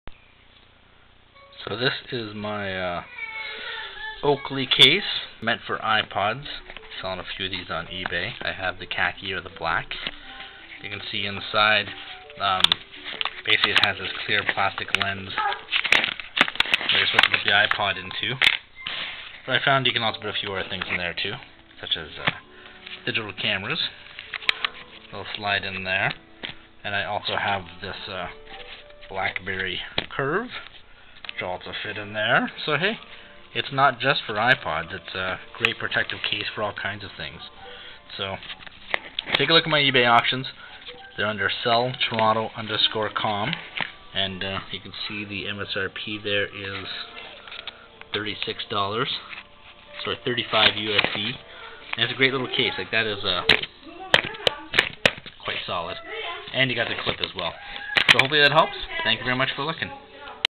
Oakley hard case for ipods that is for sale on eBay
I have quite a few of these.   They are designed for the iPod 4th gen or 5th gen video.
But in a pinch, they can hold the BlackBerry Curve or even some digital cameras as well as other MP3 players.